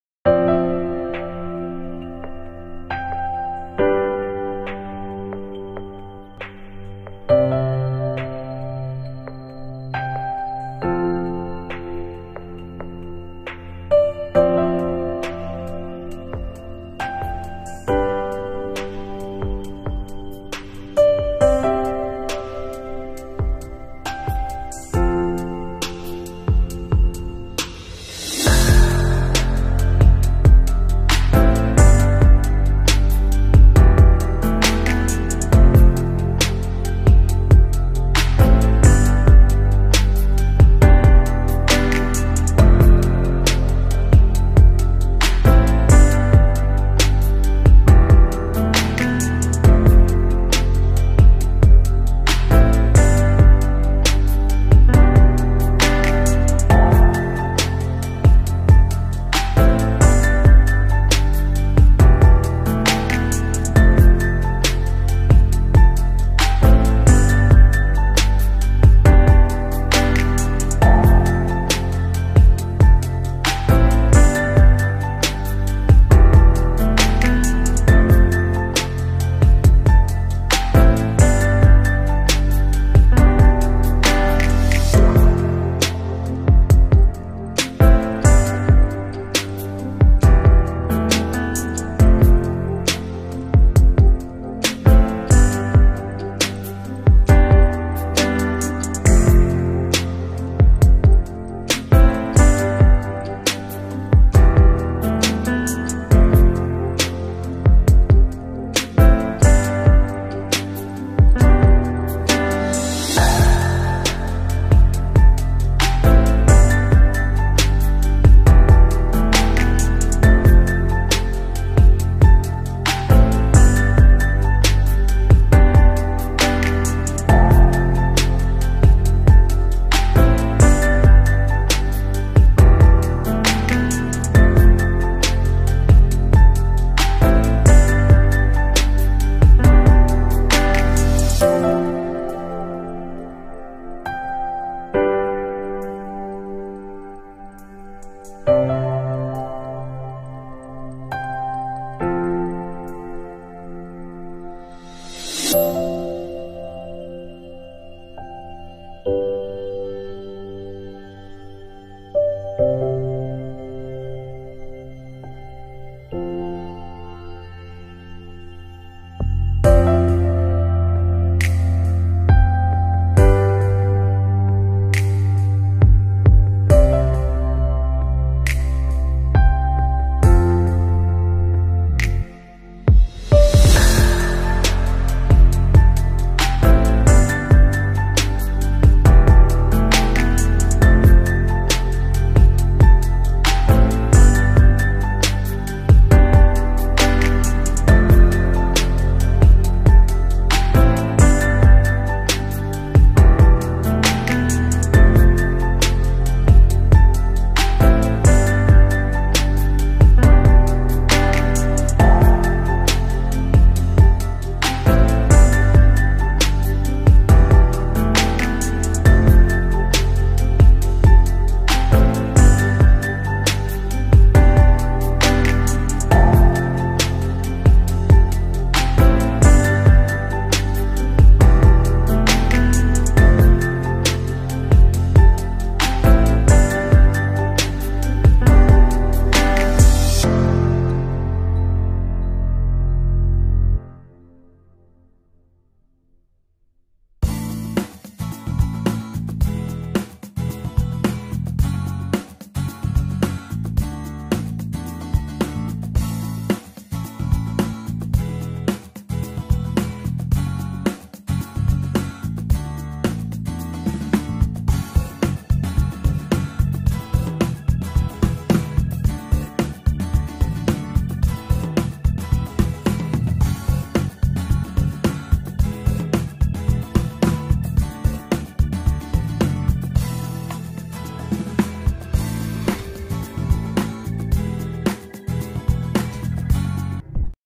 Bee's 🐝 Collecting Honey From Flowers Inside Dhaka University, Bangladesh || Campus garden
Bees collect nectar from flowers. Nectar is the sweet liquid that entices the bees to the flower. The bees climb onto or into the flower and suck up the nectar with their straw-like mouth and collect it in a little sac called a crop. They also collect pollen on their legs. As they move from flower to flower, they leave a little bit of that pollen on each new flower they visit. That's called pollination and that's how flowers reproduce.